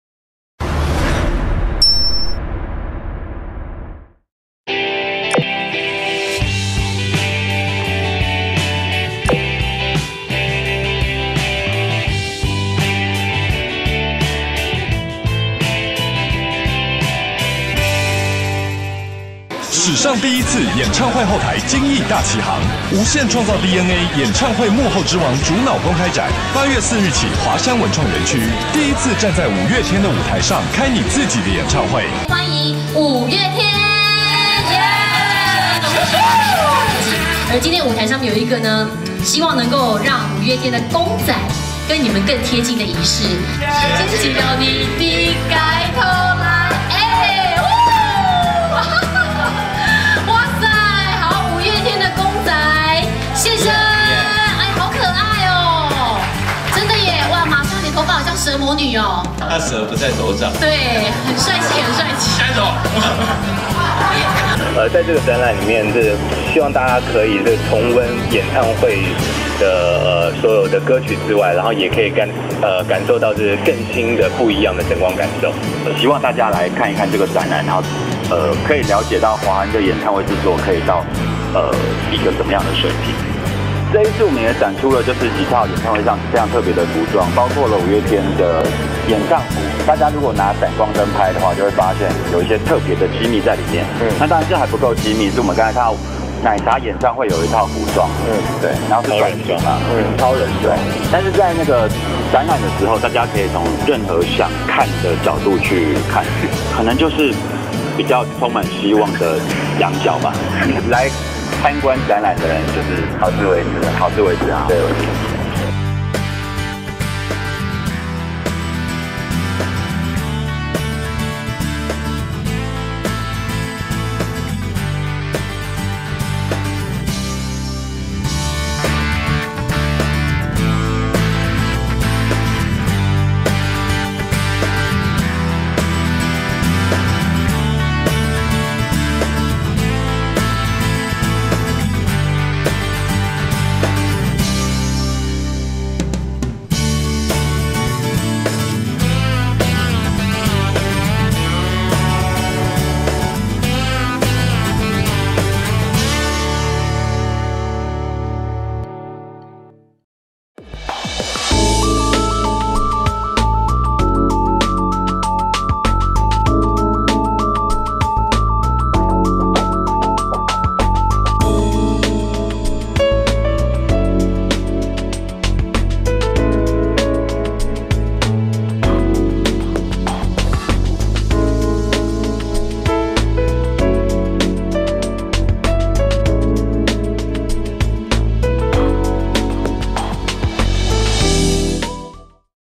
非池中藝術網 | 藝文直擊─ 華山文創園區【演唱會幕後之王主腦公開展】
由相信音樂與旺旺中時媒體集團/時藝多媒體共同策劃的「無限創造DNA 演唱會幕後公開展」即將於8月4日假華山文創園區紅磚區盛大展出，六大展區結合文化、創意、未來科技的世界級互動技術，帶你透視是誰把五月天的諾亞方舟開進了北京鳥巢？是誰又讓林宥嘉的房間飛上小巨蛋上空？ 零距離體驗演唱會之王幕後主腦用夢想的奈米對撞出一場又一場的夢幻演唱會。預購最後倒數至8月3日，全國7-11超商IBON獨家預購享DNA限定搖滾英熊票夾! 今年夏天在華山1914文創園區讓你和演唱會之王一起站上光影幻化的夢想舞臺!

五月天現身 拼湊搖滾演唱DNA
開幕記者會於華山1914文創園區紅磚六合院無限創造DNA展場舉行，特地撥空前來的五月天站在熟悉的精神指標細胞核舞台上，彷彿回到2009年DNA演唱會上滿場觀眾的搖滾感動，五位團員分別揭開由藝術家不二良親自監製高達2米的五月天Q版立體公仔！這也是不二良所有創作公仔中最大型製作，象徵DNA無限激發與放大！五座Q版立體公仔也會於展期間設置在展場外供粉絲合影拍照，記者會上團員亦分別介紹六大展區特色，帶領媒體朋友透視經累積、堆疊、激盪、變形，從無到有架構出夢想中的舞台，無數幕後工作者施展魔手幻化成真，創造一場又一場無法複製的精彩演唱會。